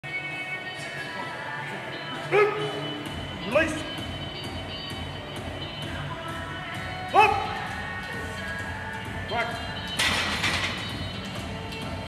705 lbs RAW Bench Eric Spoto 9/24/12 | Full Bench Workout Coming Soon | Believe Media
Eric Spoto killing a 705 lbs FULLY RAW Bench Press! Full workout video soon to come that includes Stan RHINO Efferding hitting 550 for reps!!

Date: 9/24/12